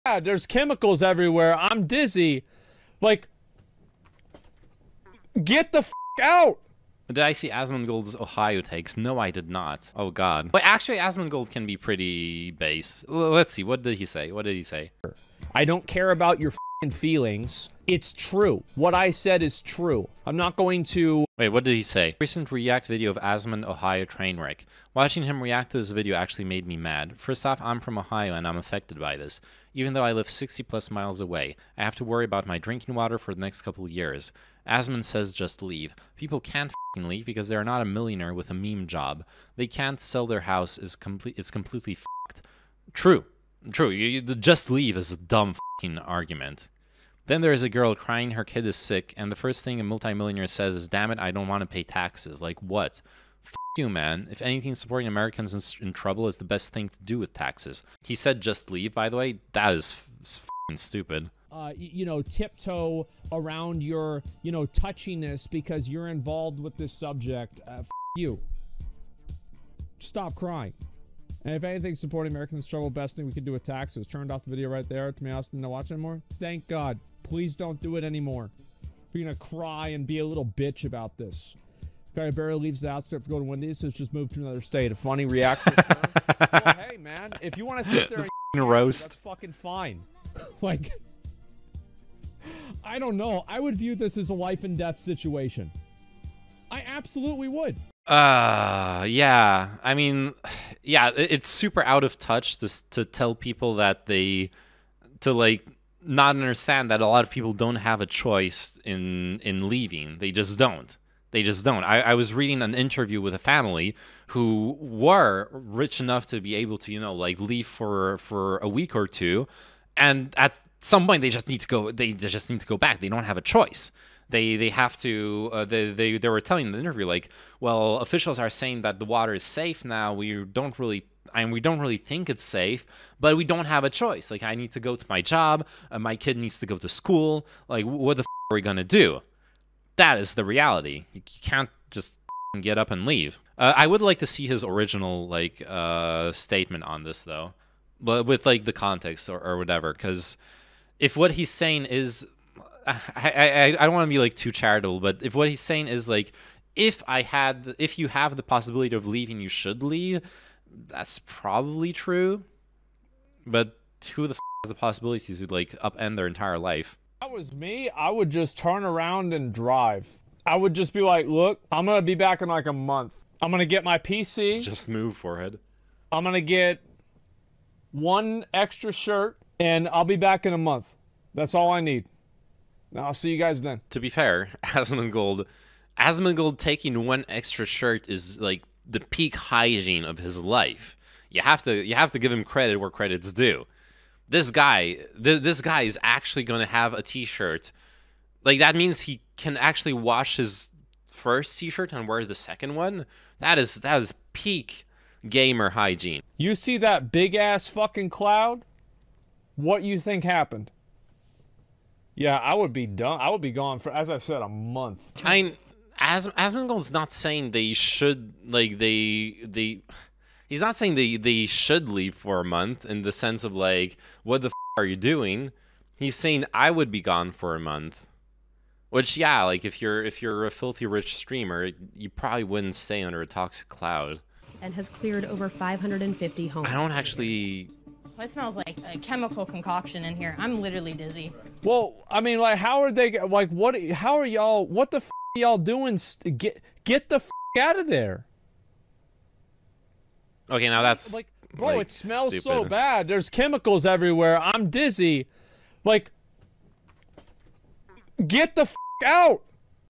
Asmongold tells people from Ohio to "Just leave"?
Reviewing some LivestreamFails drama of Asmongold, did he tell people from Ohio to just leave or is this whole thing just a clip chimp?

Timestamps:

00:00 - Coming up
00:07 - Asmongold Usually has Based takes
00:15 - Clip starts, reading the reddit post
01:00 - Clip Continues
01:37 - It's super out of touch
02:30 - I need some context
03:54 - Well... it's a stupid thing to say

06:10 - Adin Ross pressures fans to do dares for money
11:45 - The Future of Kick.com and conclusion

Edited by Knees
➤Discord Knees#3828